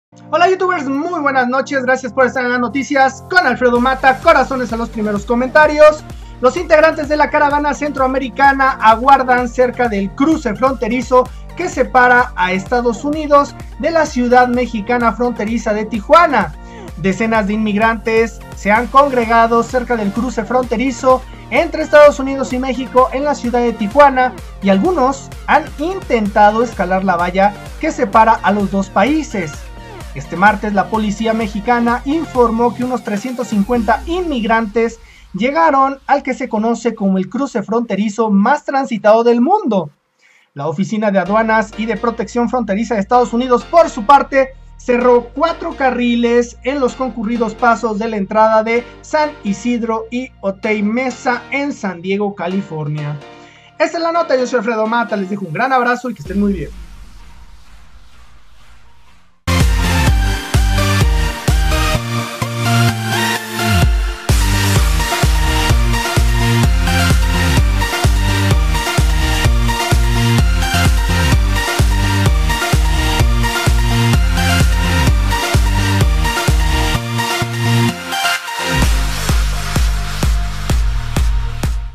Caravana Migrante No Se Atreve a Entrar a USA
Los integrantes de la caravana centroamericana aguardan cerca del cruce fronterizo que separa EE.UU. de la ciudad mexicana de Tijuana. Decenas de migrantes se han congregado cerca del cruce fronterizo entre EE.UU. y México, en la ciudad de Tijuana, y algunos han emprendido intentos de escalar la valla que separa los dos países. Este martes, la Policía mexicana informó que unos 350 inmigrantes llegaron al que se conoce como el cruce fronterizo más transitado del mundo. La Oficina de Aduanas y Protección Fronteriza de EE.UU., por su parte, cerró cuatro carriles en los concurridos pasos de entrada de San Ysidro y Otay Mesa, en San Diego (California).

~ Alfredomatta